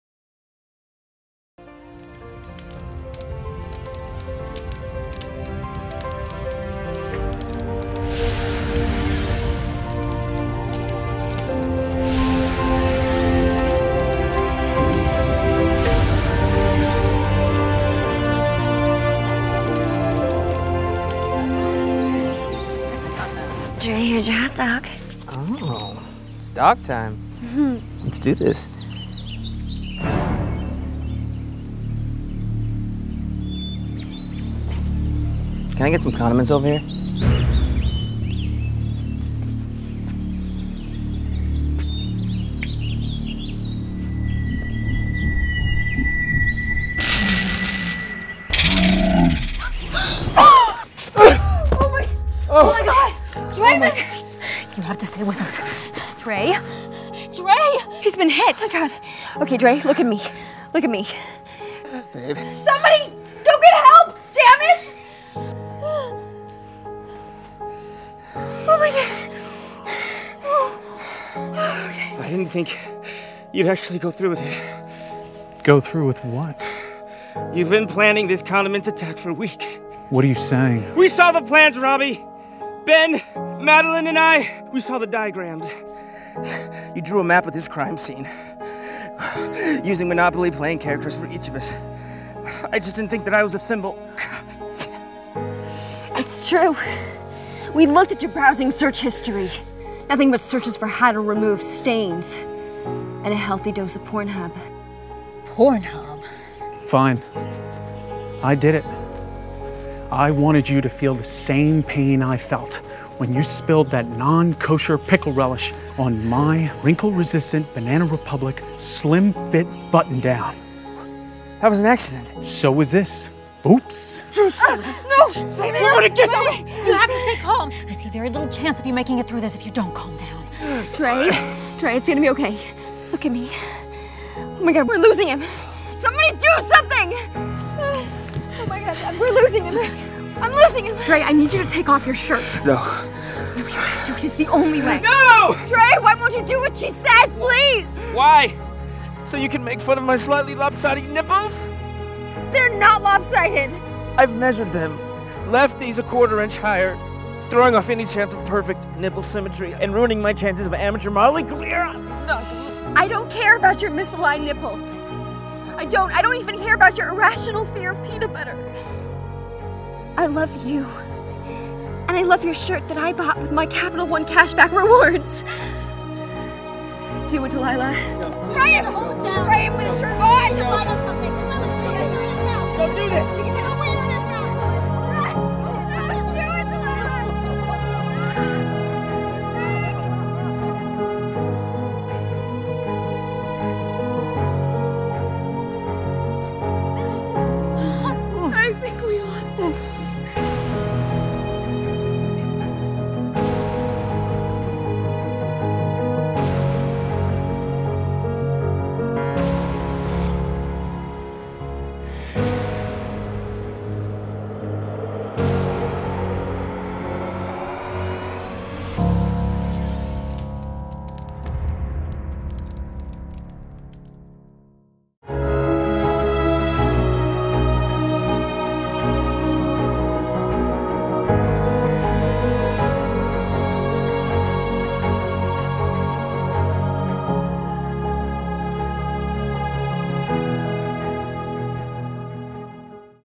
BROODING | A Stain on Society | The WC
BROODING: "A Very Serious Series" The WC brings you Brooding a new series that invites you into a world of darkness, betrayal, revenge, loss...and some other stuff. Prepare yourself for the most sensational, breathtaking, shocking, show you've ever seen.  This very serious series is all of the back end promotional content for a TV show that doesn't exist on the parody network "The WC". Each episode and promotional clip mimic the youtube content for a network TV show.

Robbie seeks his revenge on Draymond.

New promos every Friday!